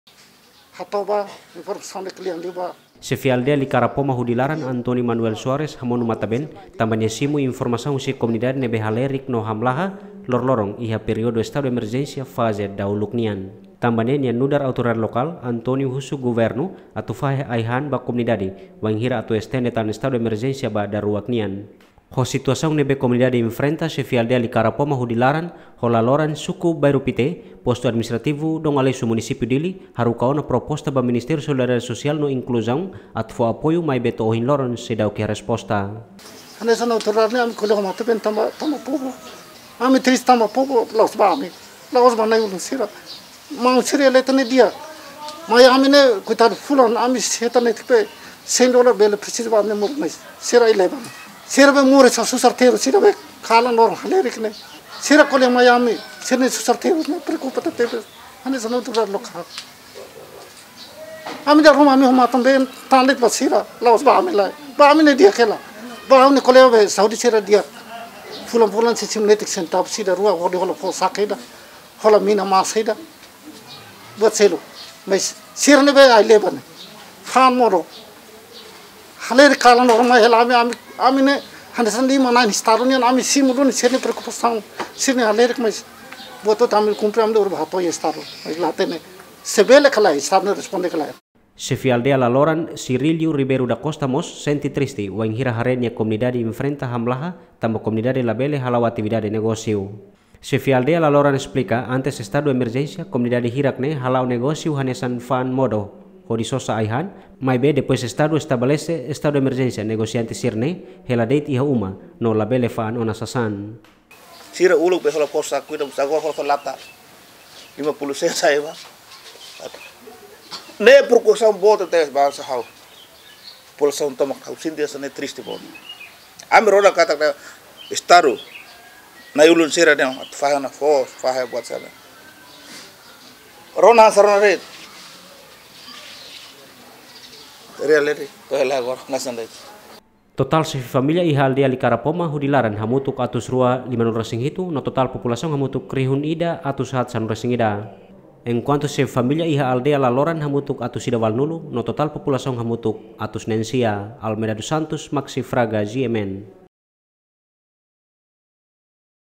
Xefe Aldeia licara-Poma Hudi Laran Antonio Manuel Soares Hamonu Matan ben
Xefe Aldeia licara-Poma Hudi Laran Antonio Manuel Soares Hamonu Matan ben tanba nia simu informasaun husi komunidade halerik no hamlaha loron-loron Durante periode Estadu Emerjensia faze dahuluk nian.